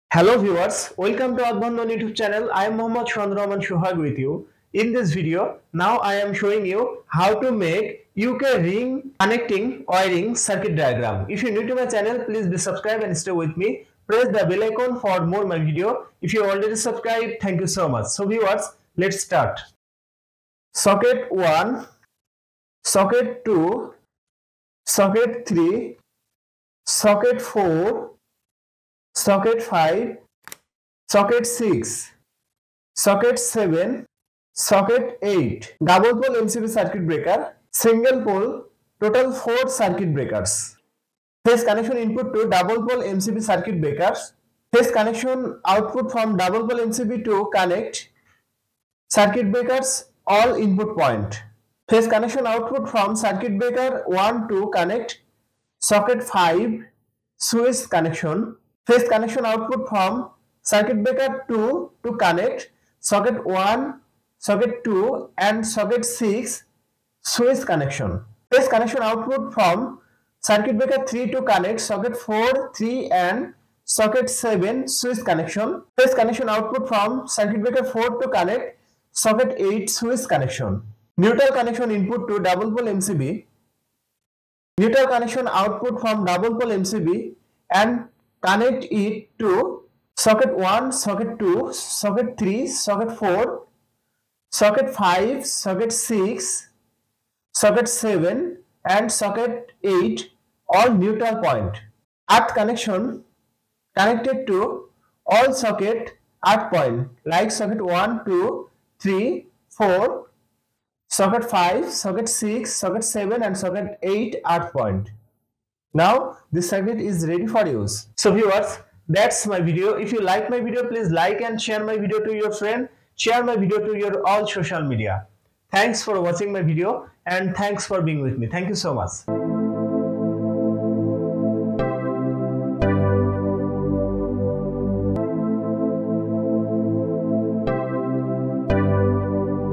How To Make UK Ring Circuit Wiring Connection Diagram | ring socket outlets
This video shows UK Ring Circuit Wiring Connection Diagram. A primarily in the UK, where it was developed, and to a lesser extent in Ireland and Hong Kong. A design enables the use of smaller-diameter wire than would be used in a radial circuit of equivalent total current capacity.